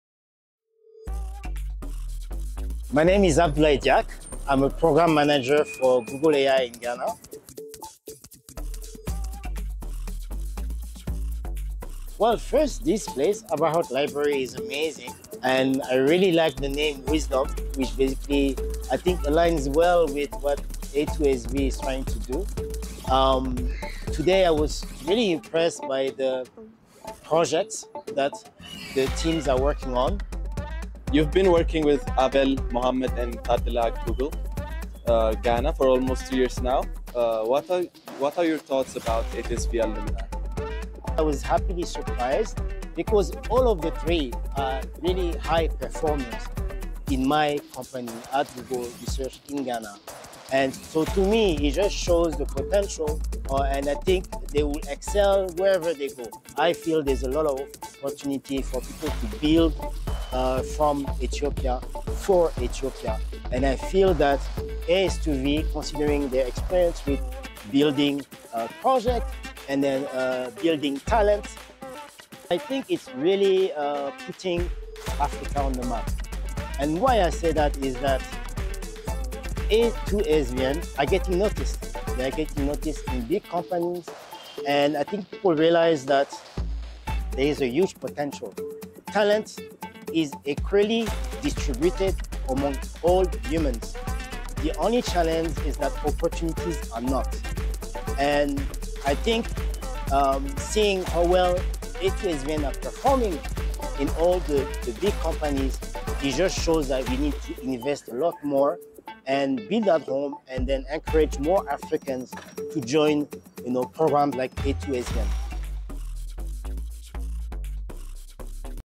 Thank You - Abdoulaye Diack
We had the pleasure of welcoming Abdoulaye Diack, Research Program Manager in AI and Machine Learning at Google Ghana!

Abdoulaye leads talent building programs and AI research initiatives, focusing on sustainable development and impact-driven innovation in Africa.

His visit started by having discussions with Generation 4 students, who are currently starting on project development phase, a key part of their second-year education at A2SV. Next, we showcased our Core Development projects, which aim to address key challenges of the continent in sectors like education and healthcare.

Finally, Abdoulaye also met with our Generation 5 students, offering insights on digital transformation in Africa and discussing the essential skills needed to excel in the tech industry.

We're grateful for Abdoulaye Diack's visit and the ongoing support from Google, enabling us to advance tech education and build digital projects for social good in Africa.

Follow our page for the latest updates and take the first step towards making a real difference with us.🌍🌟